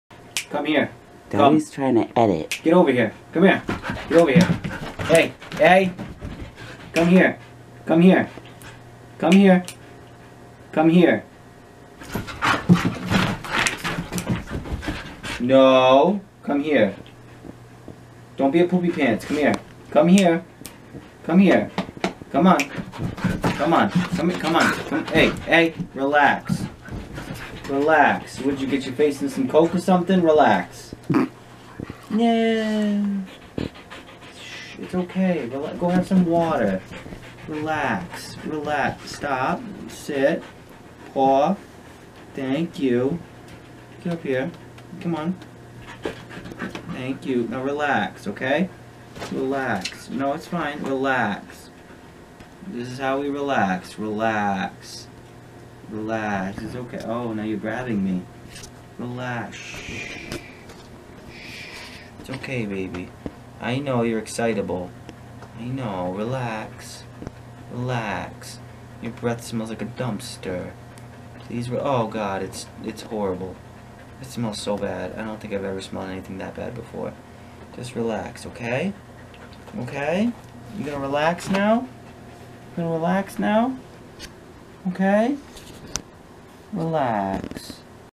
Allie Goes Nuts - Husky Fun
Our Siberian Husky Allie decides to do Zoomies in the house.  She's such a great girl!

Also check out the hashtag Siberianallie on instagram!